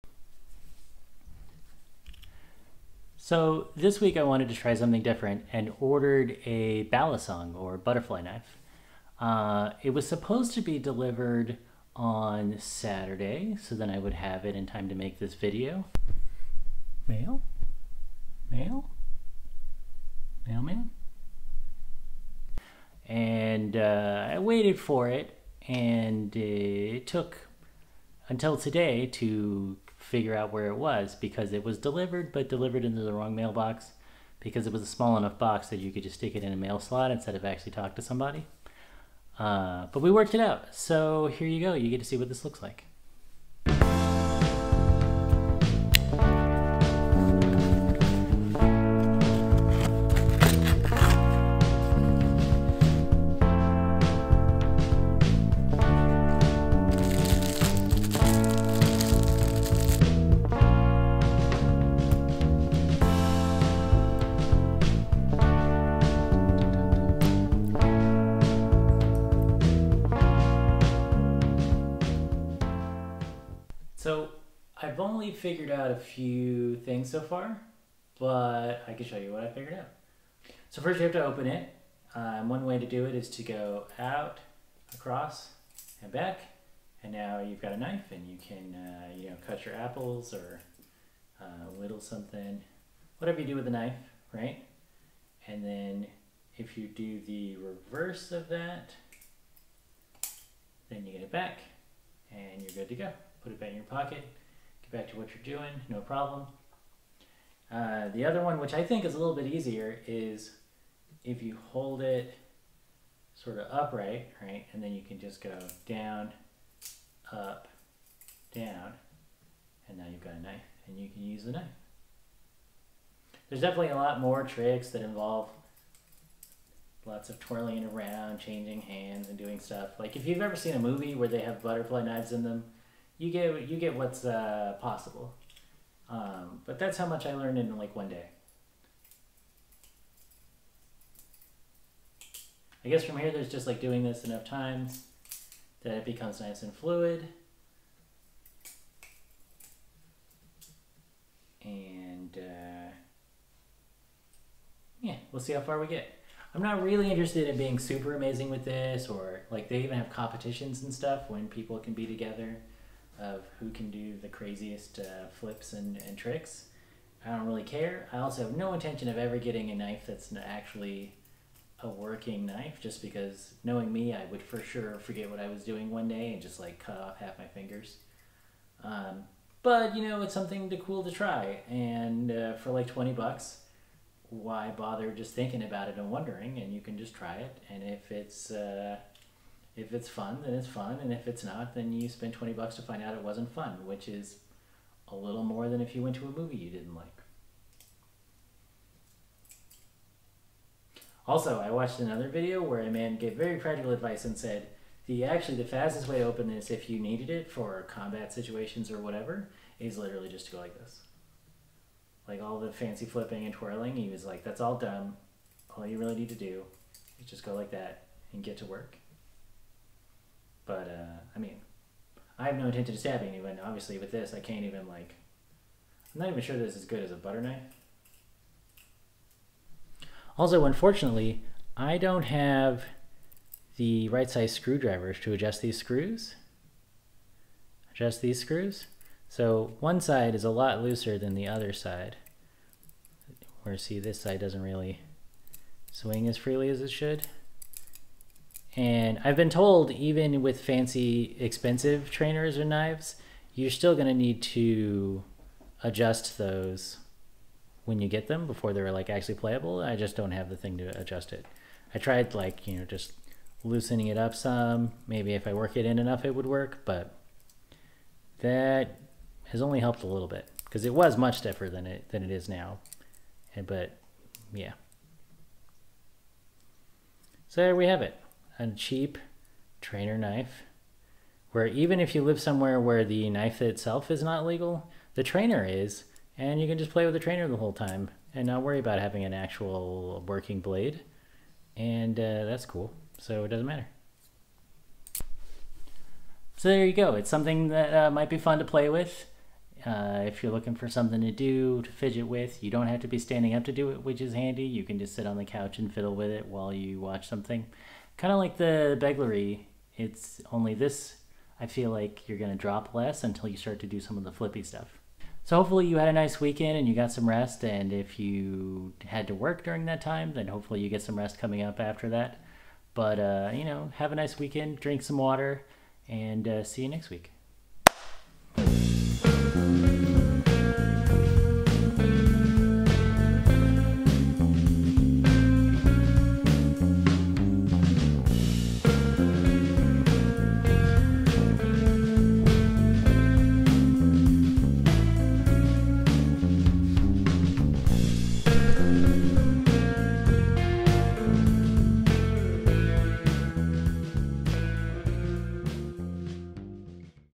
A Cheap Balisong So You Don't Have To Wonder
Ever see butterfly knives and think that may be fun, but you're not sure? I did and I just looked up a cheap one to test. It's surprisingly fun!

In this video:
Video camera: Fujifilm X-T3, Fujinon XF 18-55mm f/2.8 and  XF 16mm f1.4 lenses
Tripod: Rollei Compact Traveller Carbon
Yoyo: Yoyofactory Meerkat, Airetic String